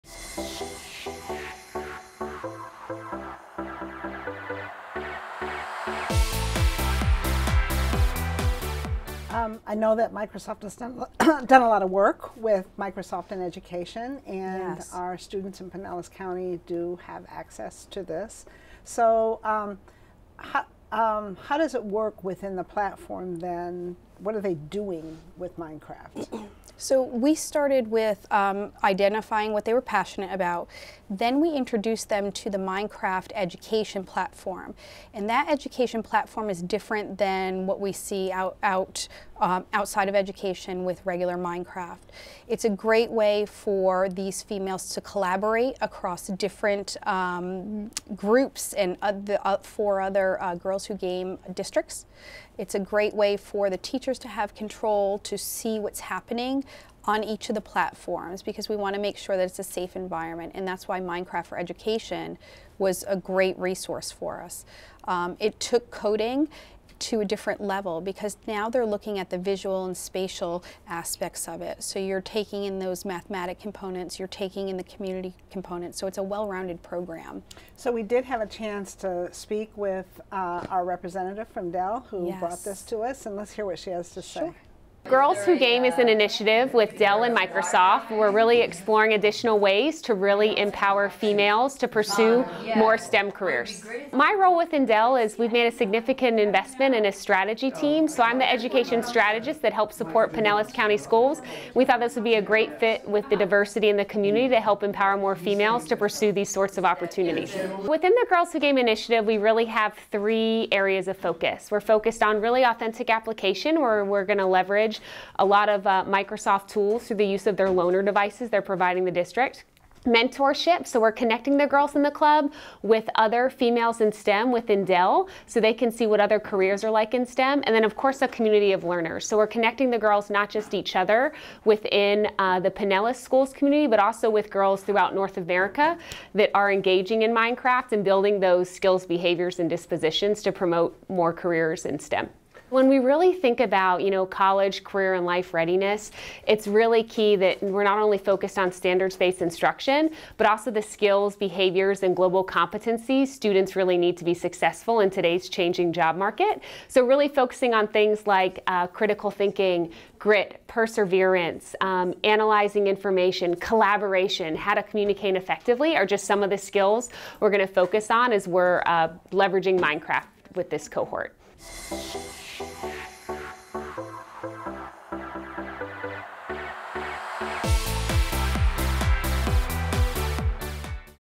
Digital Learning 363 YT Girls Who Game
Host Connie Kolosey and guest Laura Spence discuss the benefits of the Girls Who Game program in Pinellas County Schools.